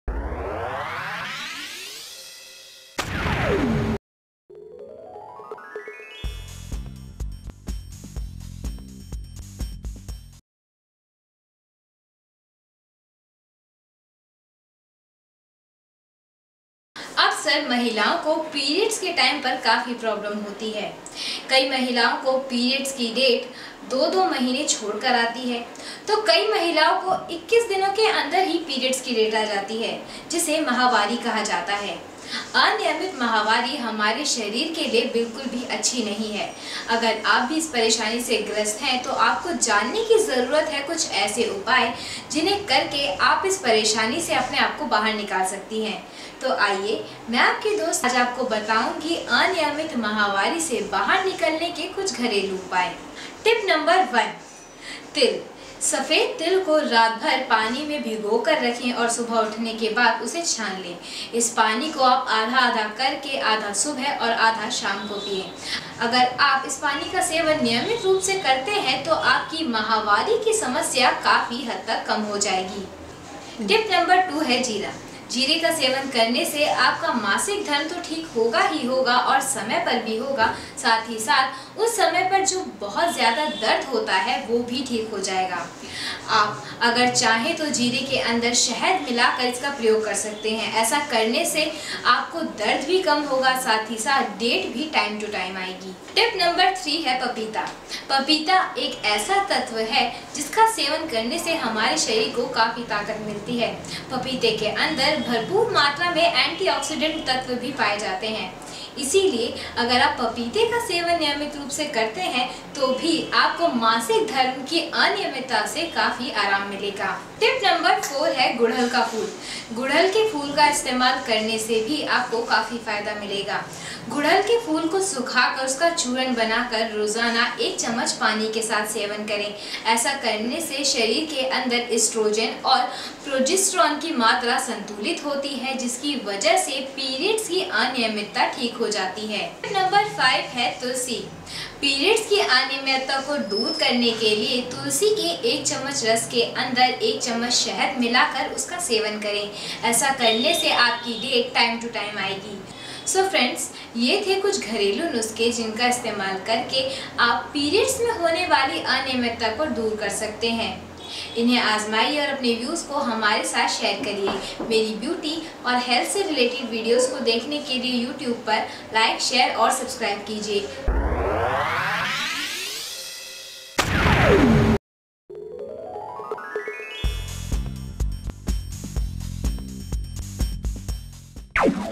पीरियड जल्दी लाने के आसान उपाय | Mahavari Lane Ke Upay in Hindi
पीरियड जल्दी लाने के आसान उपाय | Mahavari Lane Ke Upay in Hindi aksar mahilayo ko periods ke time par kaafi problems hoti kyi mahilayo ko periods ki date do do mahine chod kar aati hai to kyi mahilayo ko 21 din ke andar hi periods ki date aa jati hai jisse mahavari kha jata hai aniyamit mahavari humare sharir ke liye bilkul bhi acchi ni hai agar ap bhi is preshani se garsth hai to apko jaane ki jarurt hai kuch aise upaye jinhe karke ap is preshani se apne apko bahar nikal skti hai to dosto aj jante hai ki aniyamait mahavari se bahar nikalne ke luch gharelu upaye. पीरियड जल्दी लाने के आसान उपाय | Mahavari Lane Ke Upay in Hindi